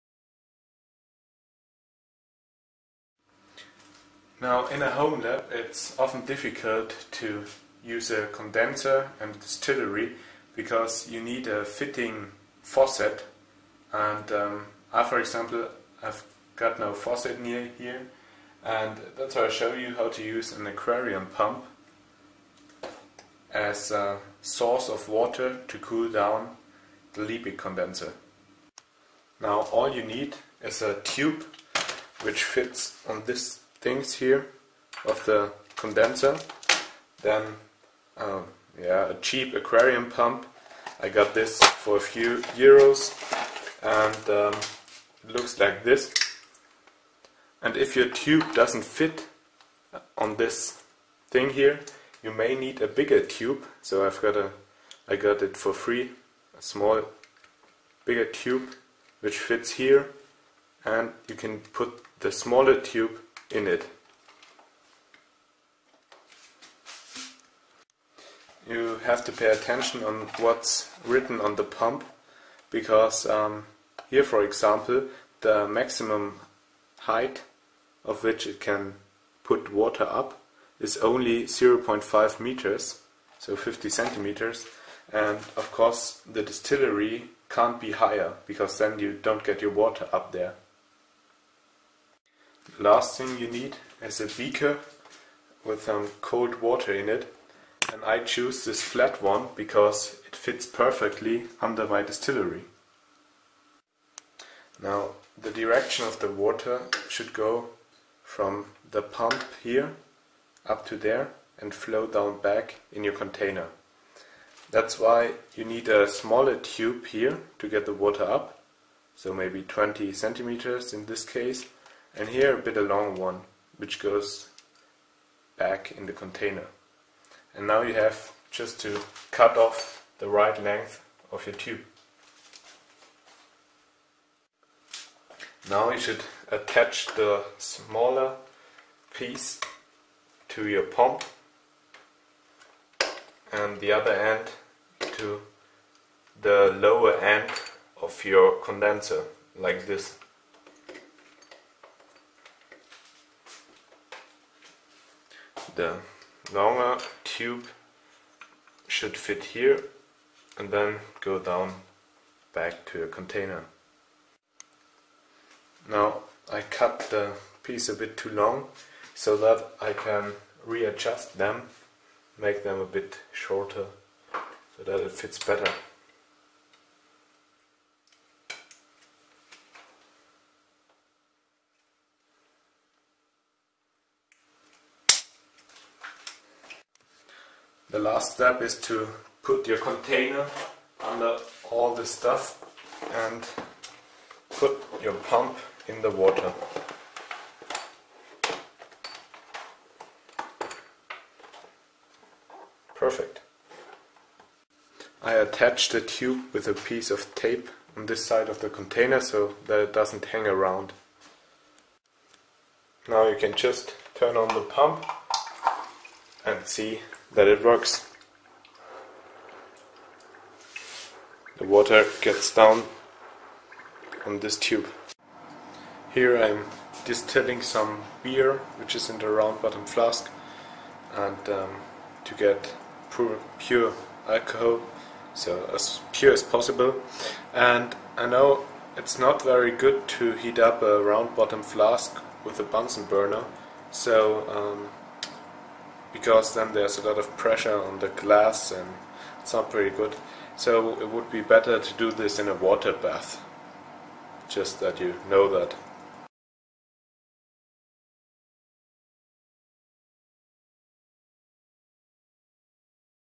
How to Arrange a Portable Water Current System for a Distillery
In small home labs it´s often difficult to work with a distillery because a faucet for the condenser may not be available.

Today I´ll show you how to build your own small and cheap water current system for a distillery.

You only need a aquarium pump (5W should be enough), a tube which fits on the extension pipes of your condenser as well as on the pump and finally a container filled with cold water. instagram: floka_98